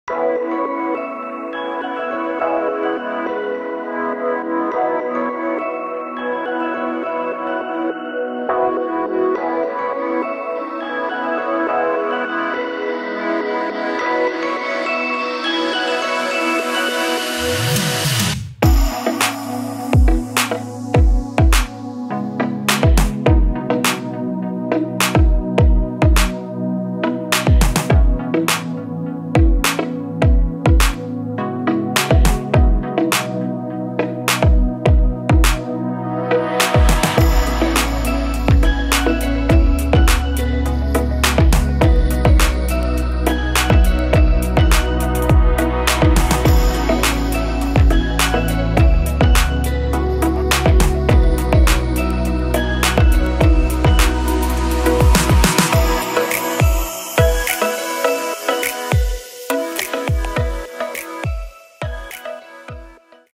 Unboxing: Sada videotelefonu EMOS EM-05R s ukládáním snímků | EMOS H2030
✅ Požadovaná kabeláž - FTP nebo SYKFY 5 × 2 × 0,5
✅ Rozměr kamerové jednotky - 55 × 40 × 130 mm
✅ Rozměr monitoru - 204 × 26,5 × 144 mm
✅ Přísvit kamery - do 150 cm
✅ Krytí IP 44

Zajímá Vás celá naše řada videotelefonů?